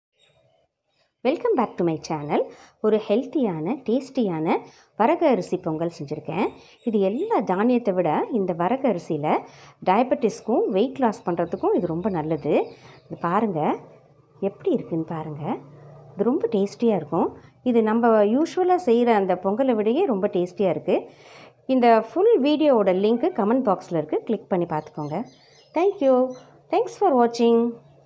Varagu Arisi Pongal / Kodo Millets Pongal.
Ingredients :-
வரகு அரிசி / Varagu Arisi - 1 Cup
பாசிப்பருப்பு / Moong dal - 1/2 Cup
தண்ணீர் / Water - 4 Cups
நெய் / Ghee - 2 Tbsp
எண்ணெய் / Oil - 2-3 Tbsp
மிளகு / Pepper - 1 sp
சீரகம் / Cumin seeds - 2 sp
இஞ்சி / Ginger - 2 inches chopped
பச்சை மிளகாய் / Green Chilies - 2
கொத்தமல்லி கறிவேப்பிலை / Coriander Curry leaves
முந்திரி பருப்பு / Cashew nuts

For Coconut Chutney:-
Fresh Coconut - 1 Cup
Fried gram - 1 Tbsp